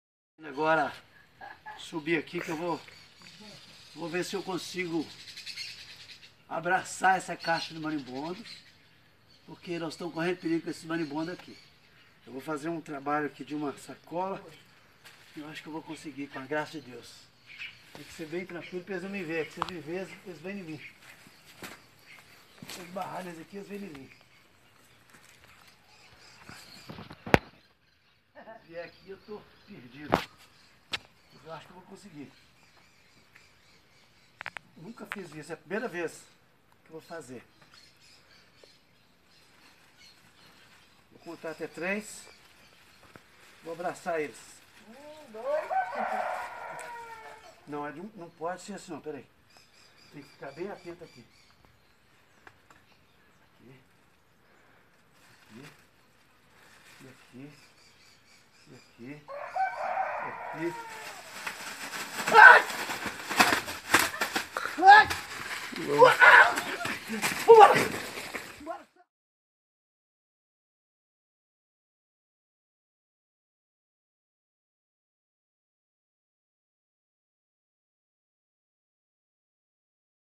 Man tries to remove wasp nest with plastic bag and fails horribly - Daily Mail
This man decided to avoid the professionals and take it upon himself to remove a wasp nest from a tree. But it is safe to say he definitely doesn't have it in the bag. He was filmed climbing a ladder in his backyard to tackle the nest armed with nothing but a green plastic shopping bag. The man chatted away calmly as he prepared to smother the wasps inside the nest, but predictably things didn't go to plan.